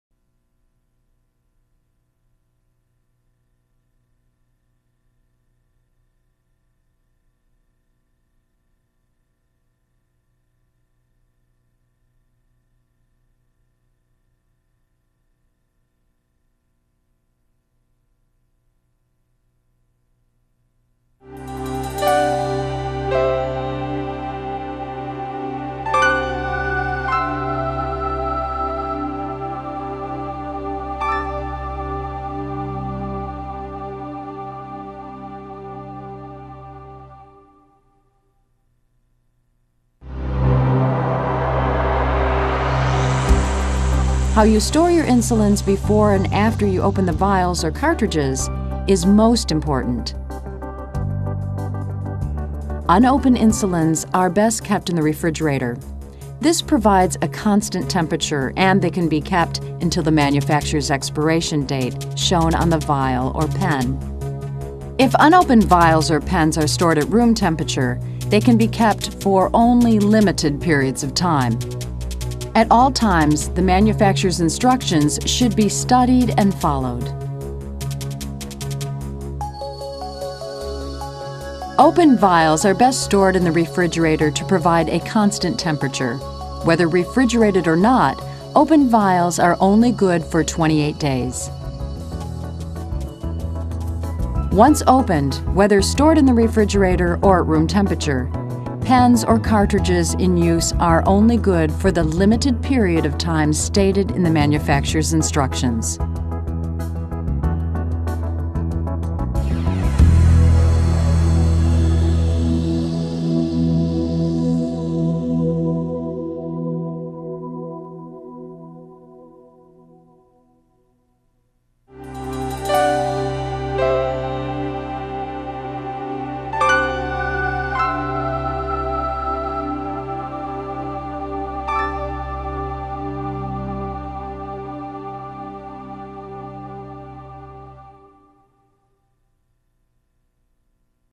Diabetes Patient Education Insulin Storage
Diabetes Patient Education
How you store your insulins before and after you open the vials or cartridges is most important.
Unopened insulins are:

    * best kept in the refrigerator.
    * This provides a constant temperature
    * and they can be kept until the manufacturer's expiration date shown on the vial or pen.
Diabetes Patient Education
    * If unopened vials or pens are stored at room temperature, they can be kept for only more limited periods of time.
    * At all times, the manufacturer's instructions should be studied and followed.

Opened vials are:
Diabetes Patient Education
    * best stored in the refrigerator to provide a constant temperature.
    * Whether refrigerated or not, opened vials are only good for 28 days.

    * Once opened, whether stored in the refrigerator or at room temperature, pens or cartridges in use are only good for the limited period of time
    * stated in the manufacturer's instructions.

Diabetes Patient Education